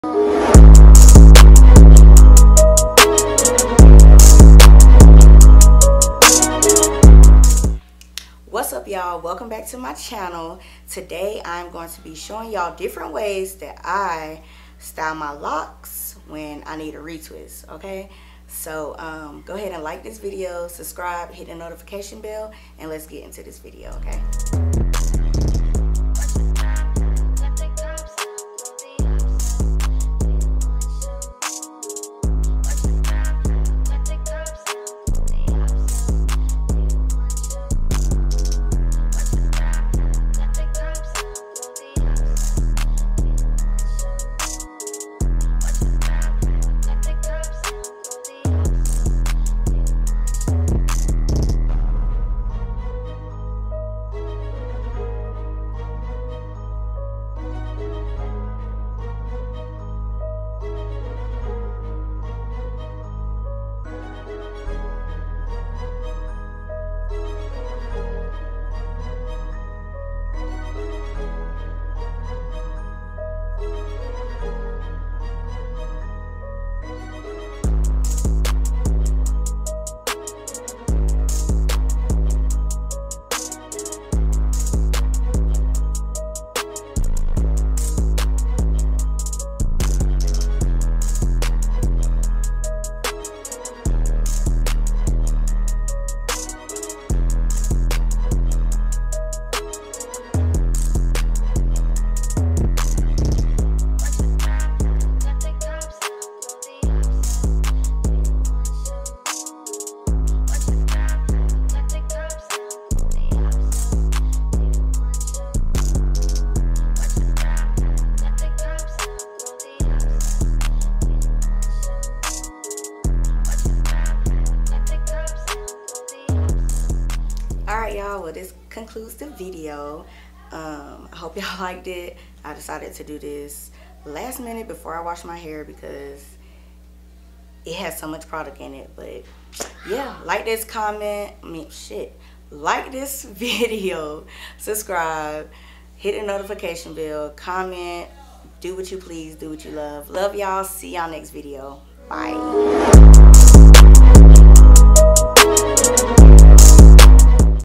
6 Ways To Style Your LOCS!
Hey, Hey, Hey! Here are 6 different ways I style my Locs when I need a retwist real bad! Lol which is all of the time. I usually retwist my hair every 4 to 6 months! I love the frizzy look! I hope y'all all enjoy these different hair styles!

Tiktok.com/Evyevette